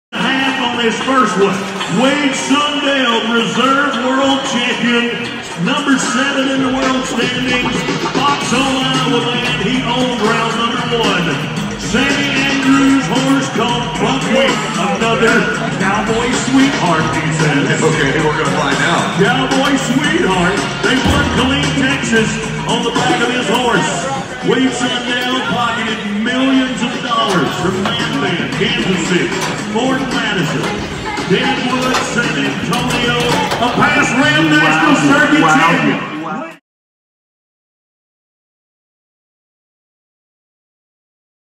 Wade Sundell & Sammy Andrews In the Chutes
Watch owner Sammy Andrews help Cowboy Sweetheart and Wade Sundell get a safe, clean start to their contest.